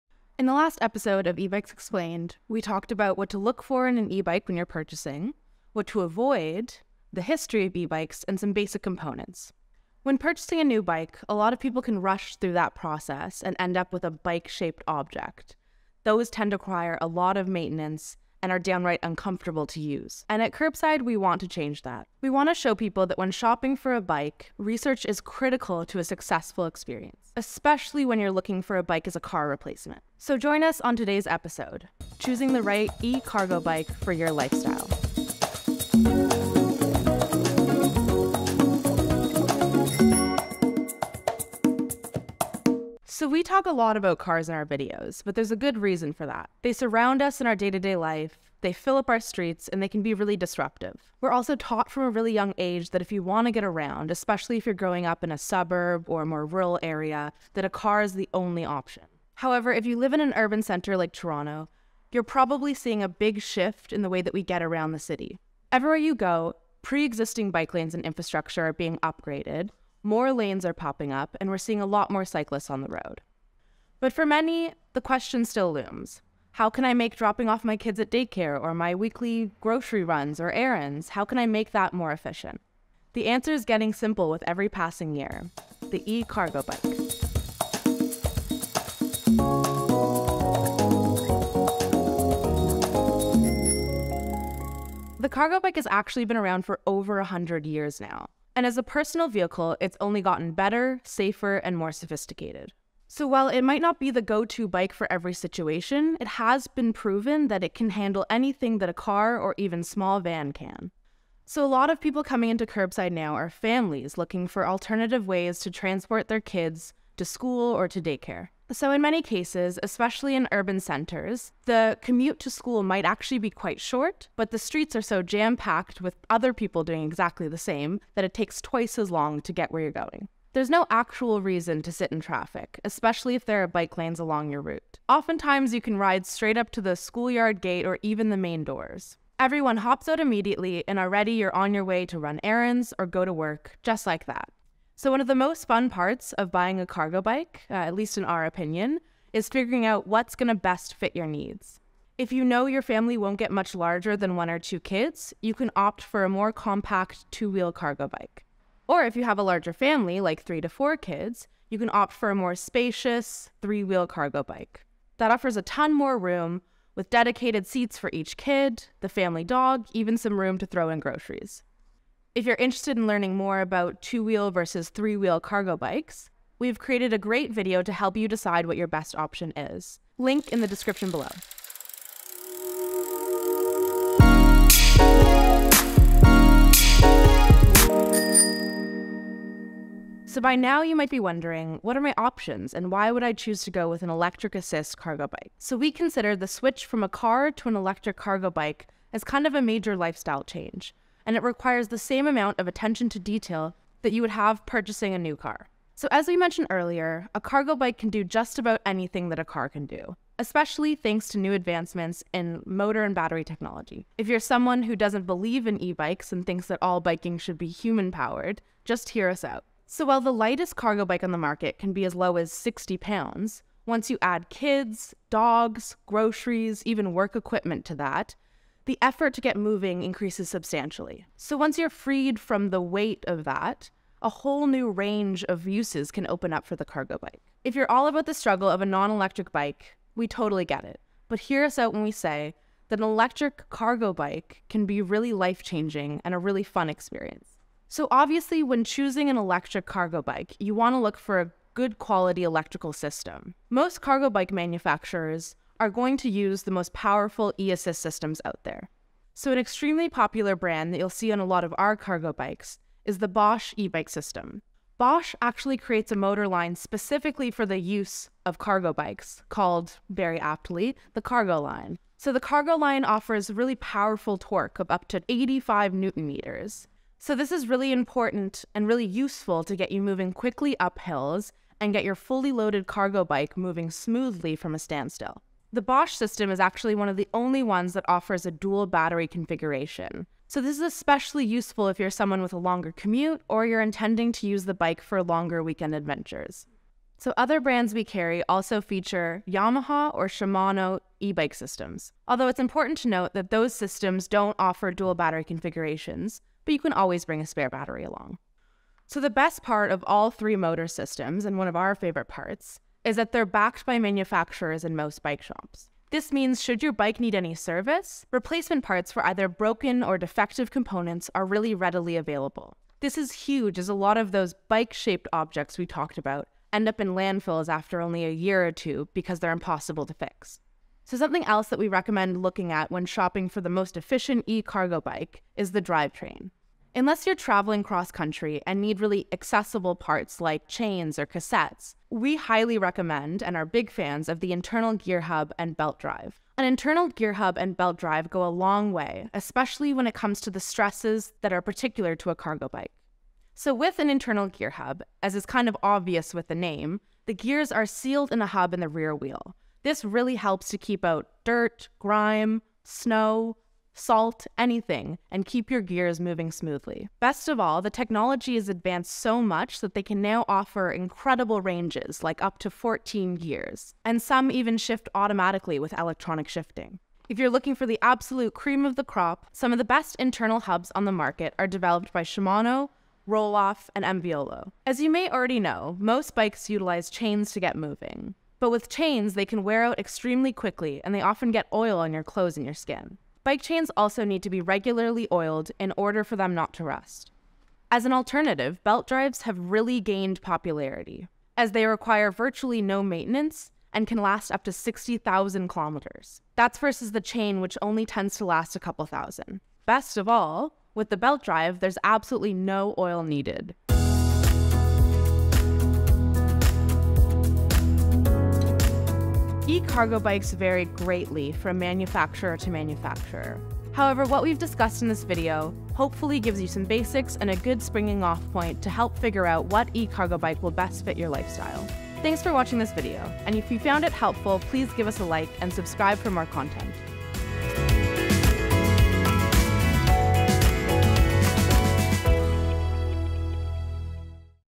How to Choose Your E-Cargo Bike
In this video we cover some of the very basics on what you might need on an electric assist cargo bike. If you want deeper detail in 3-wheeled vs 2-wheeled and carrying capacity and some accessories check out our video: Cargo Bike Face-Off!